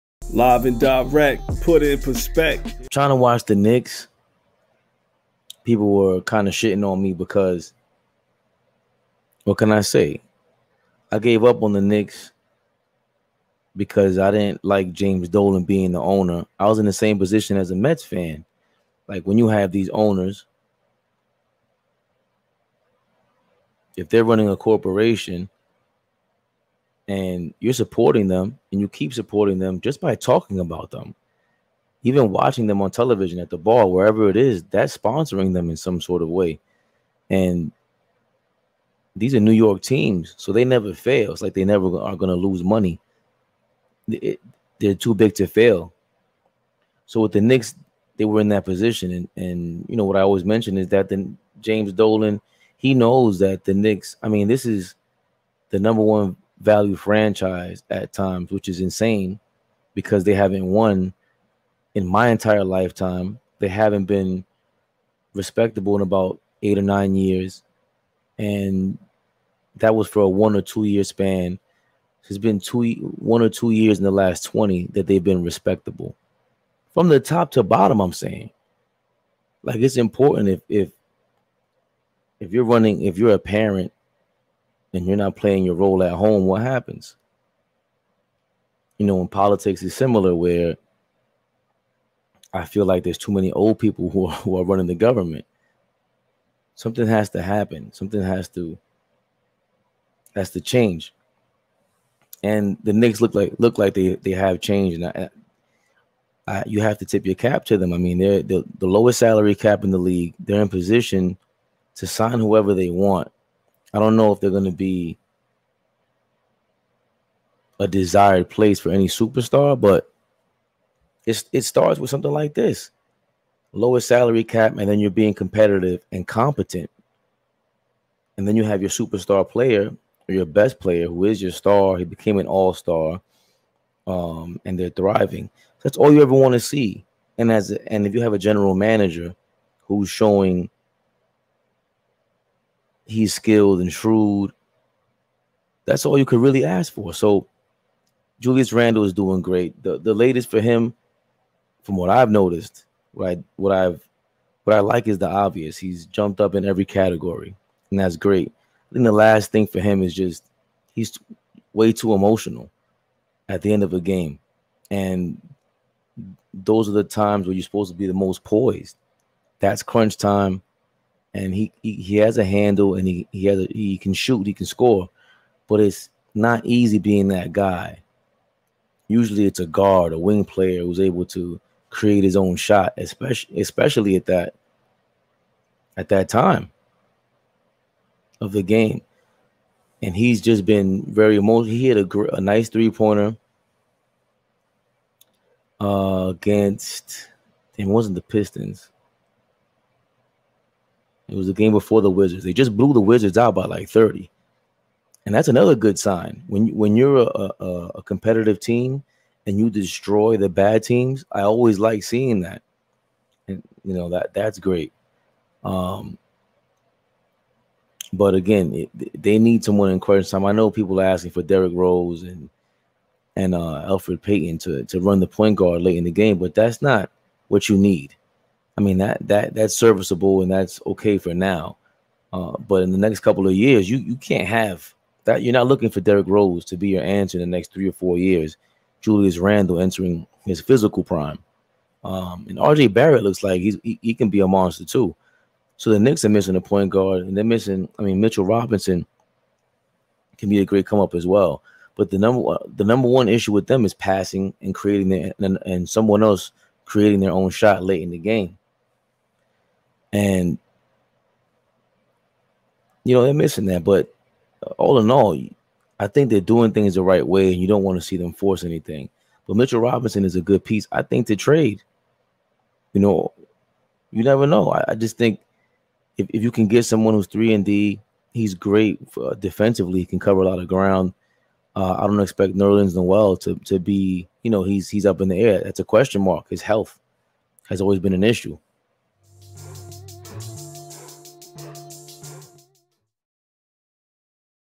KNICKSTAPE 3/25/21
Raymundo (@fabian_perspectv) speaks on the current state of the Knicks.

00:51 RAYMUNDO EXPLAINS WHY HE GAVE UP ON THE KNICKS BUT HOW THEY'VE PROVEN THEMSELVES 😒

02:28 JULIUS RANDLE, THE GOOD 🤠, THE BAD ☹

03:38 WHAT DO THE KNICKS NEED?

Putitnperspectv.com

FACEBOOK PAGE; Putitnperspectv